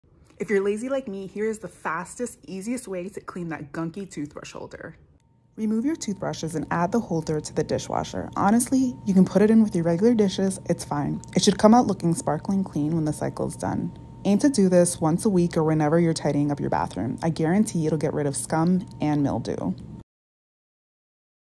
Clean Your Toothbrush Holder With This Easy Hack
Clean your toothbrush holder once a week with this easy cleaning hack!

Sign up for our newsletters if you want more ways to simplify your life